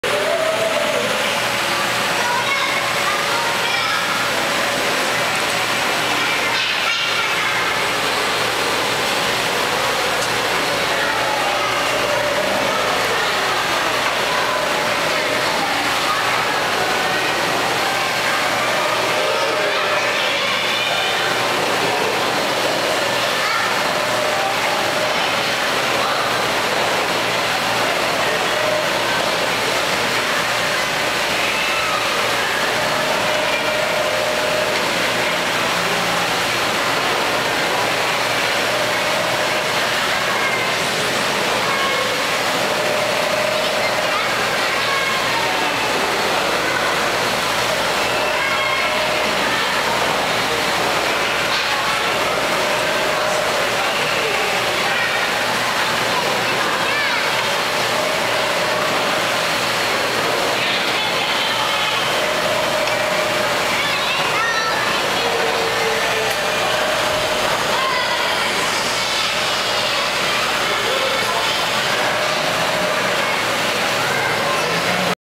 Furry Go Round
Furries riding the Binda Conservation Carousel at Binder Park Zoo.